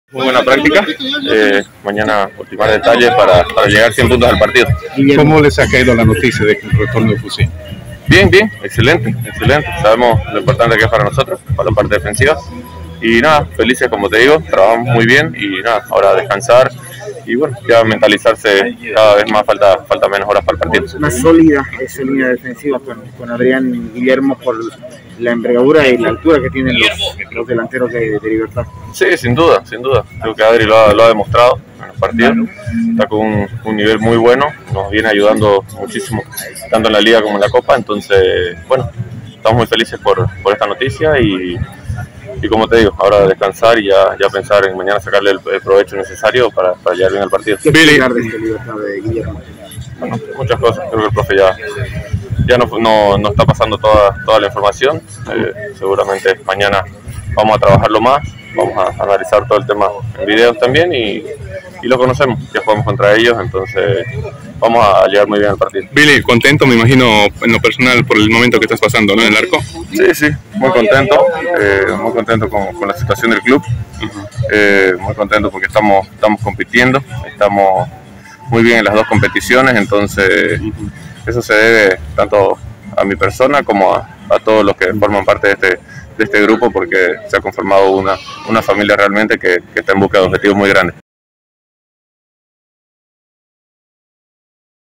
EN LA CANCHA DE OLIMPIA
TheStrongest trabajó este martes en la cancha de Olimpia, habla el BillyVizcarra del partido que se viene este jueves. Más información en CompetenciaBolivia de lunes a viernes a las 13 y 19 hrs.